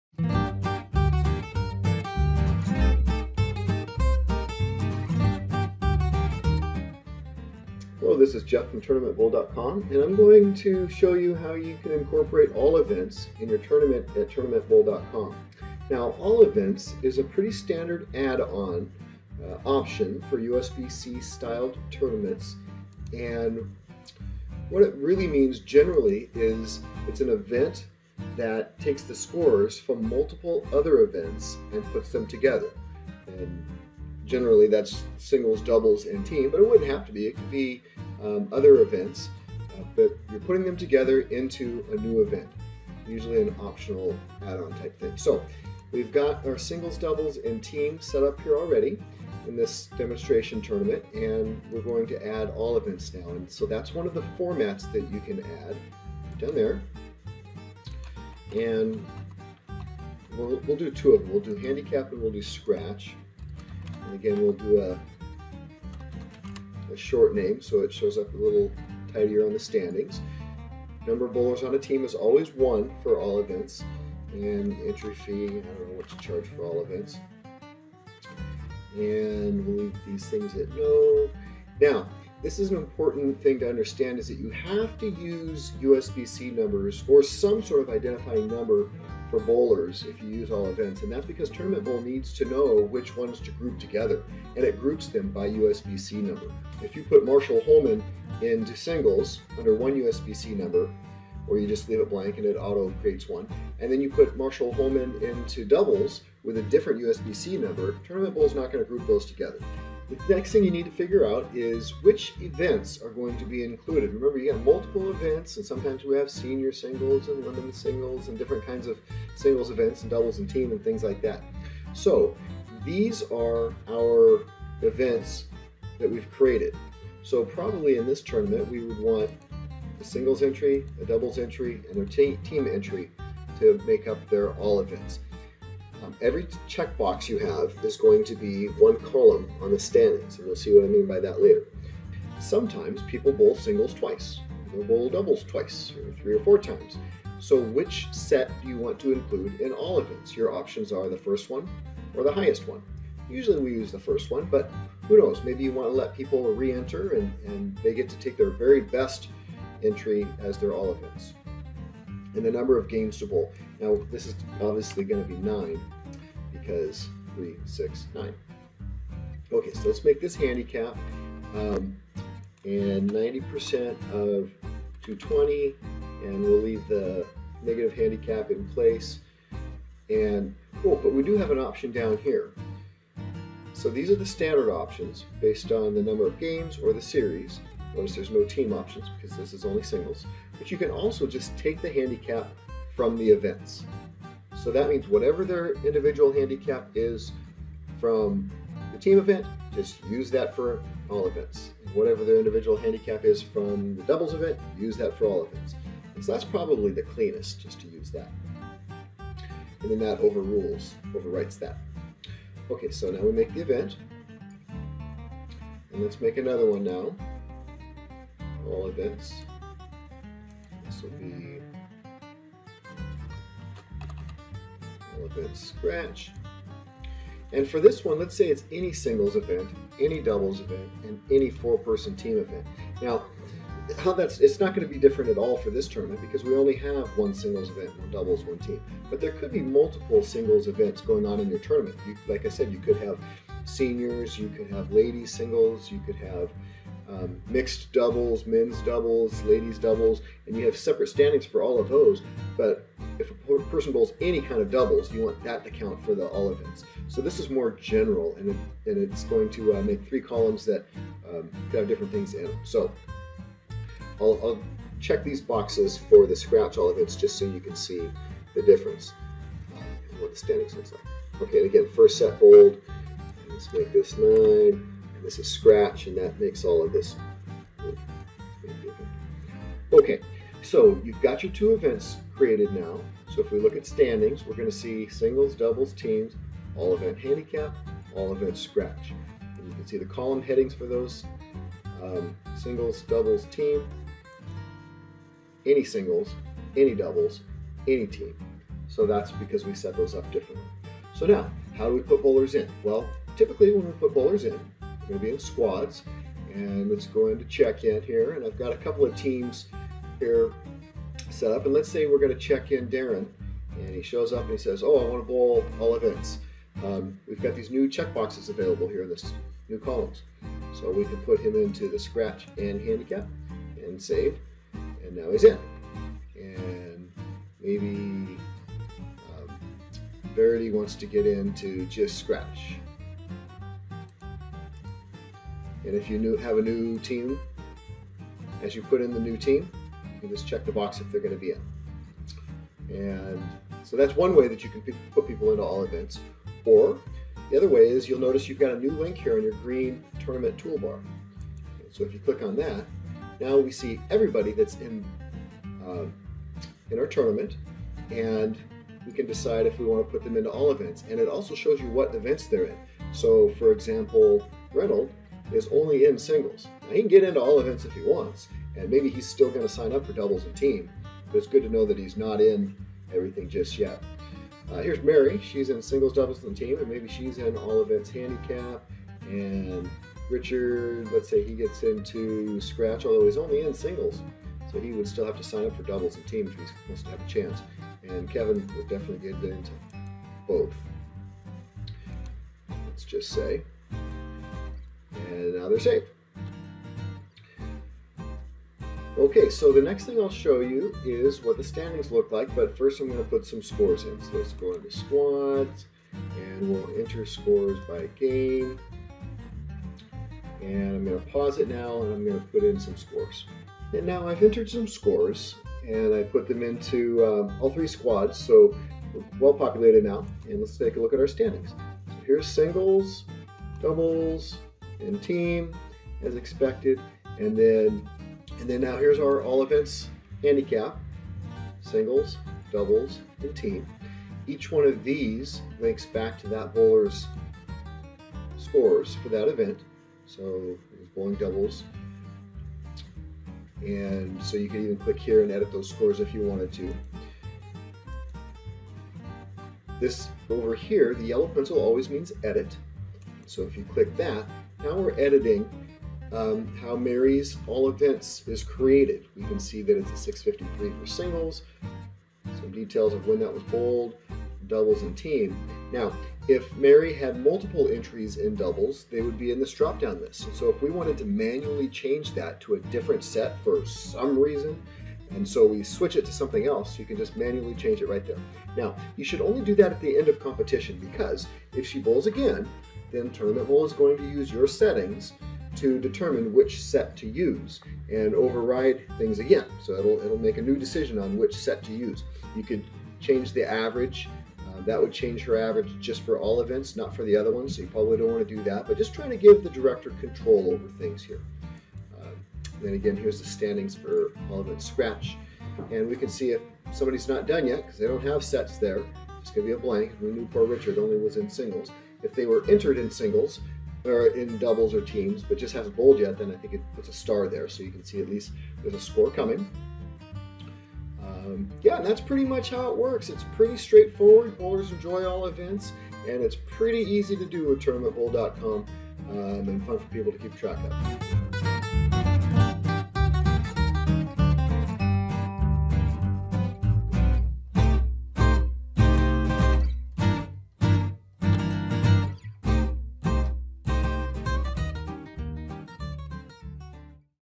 TB Tutorial All Events
This video will show you how to create an "all events" format event.  This means scores from multiple events will be combined into one event.